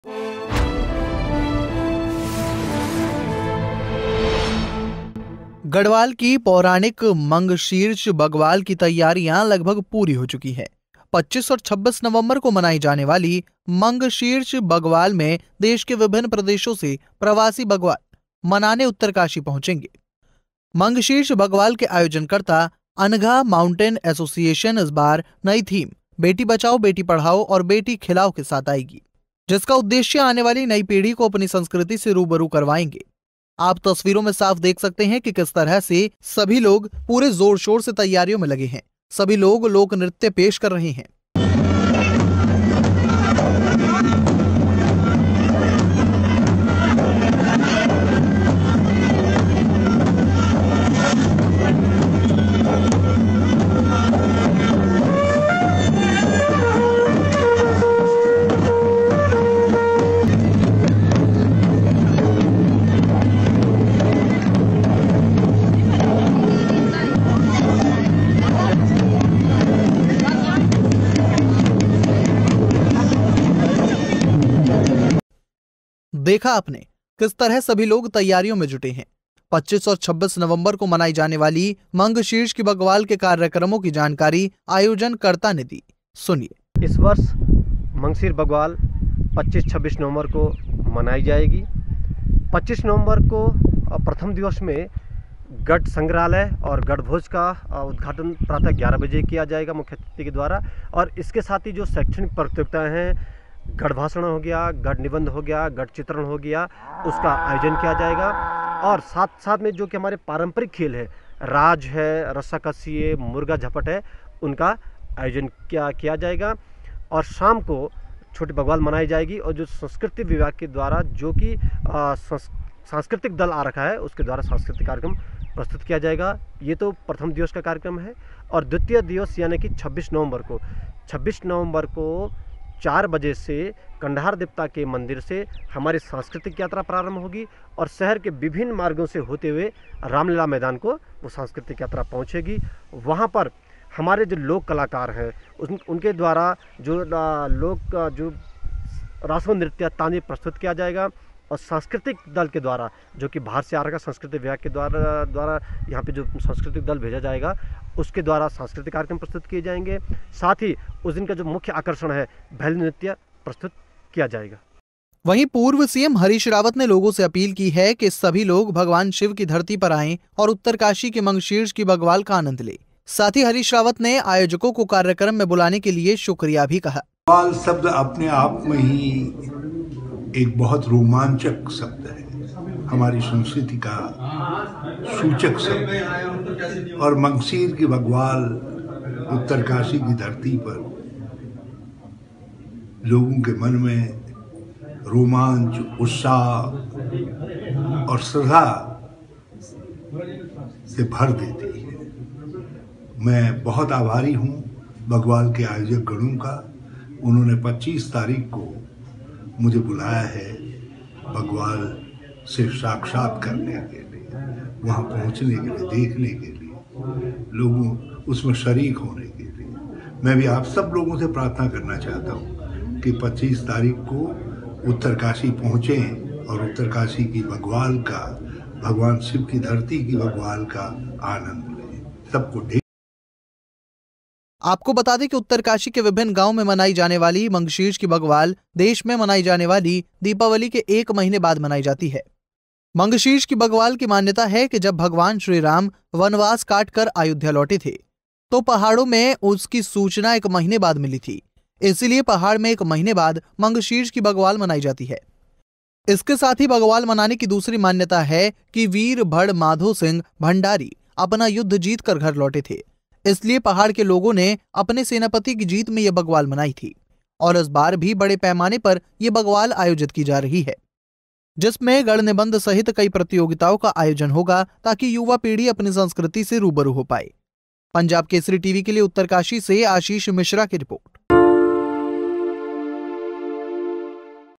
उत्तरकाशी में मंगशीर्ष बग्वाल की तैयारियां पूरी, देंखे क्या है इस बार बग्वाल में खास
गढ़वाल की पौराणिक मंगशीर्ष बग्वाल की तैयारियां लगभग पूरी हो चुकी है...25 और 26 नवंबर को मनाई जाने वाली मंगशीर्ष बग्वाल में देश के विभन्न प्रदेशों से प्रवासी बग्वाल मनाने उत्तरकाशी पहुंचेगे.